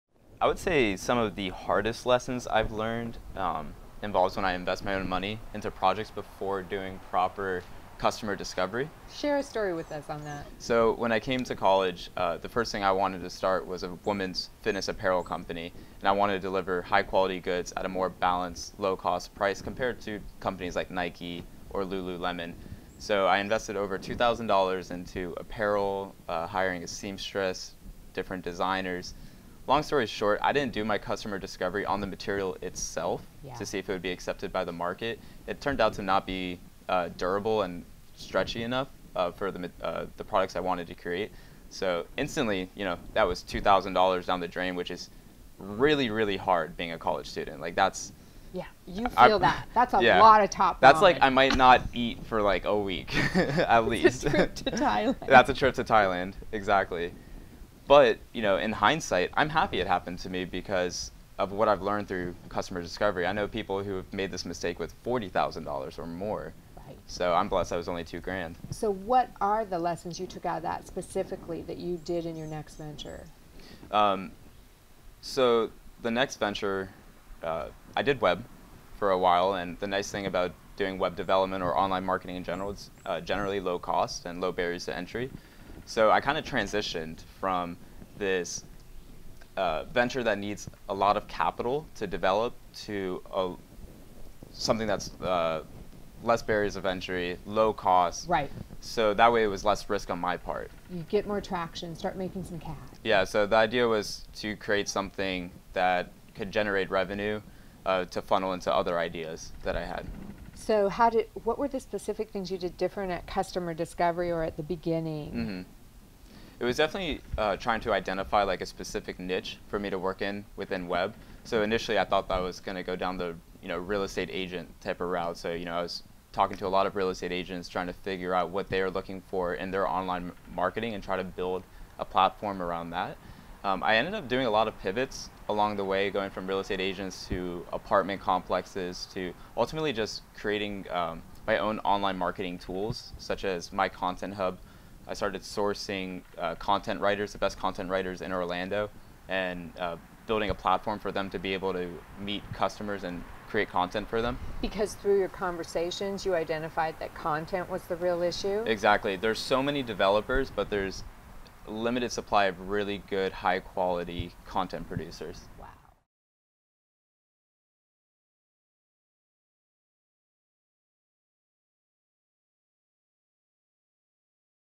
The Thing I Won't Do Again When I Start My Next Business : David Freund
David Freund started his first money making venture at age 3 and has been at it ever since. In this excerpt of his Dear Rockstar interview listen as he shares his hardest lesson and why he's a believer in customer discovery before you start...now.
Listen to the entire conversation between Pam Hoelzle and David Freund from Globalized Citizen